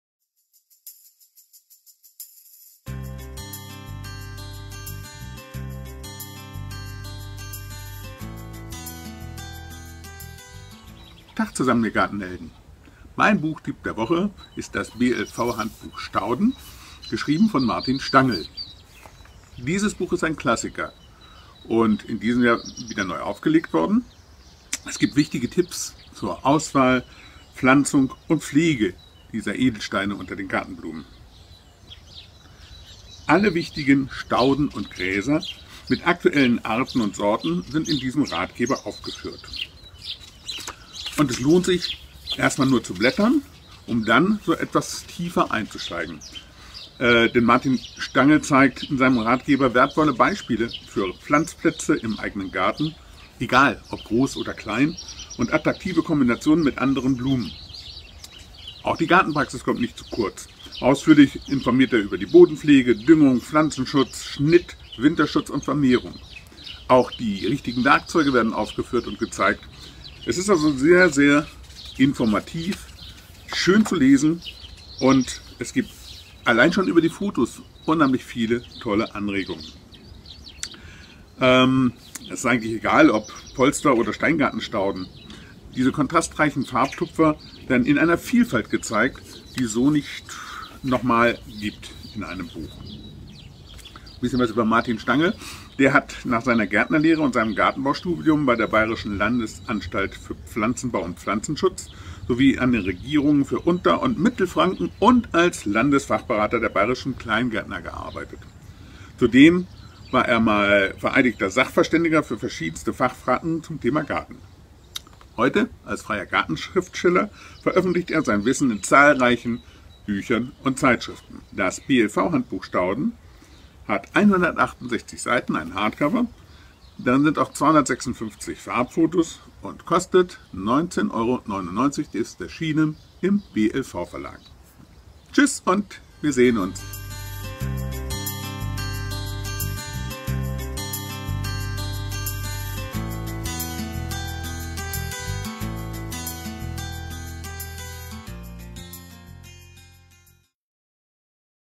BLV Handbuch Stauden - gartenHELDEN.de Buchtipp der Woche 15/2014
Stauden finden in jedem Garten ihren Lieblingsplatz. Martin Stangl zeigt in seinem Ratgeber wertvolle Beispiele für Pflanzplätze im eigenen Garten. gartenHELD Manfred Eckermeiers Buchtipp der Woche.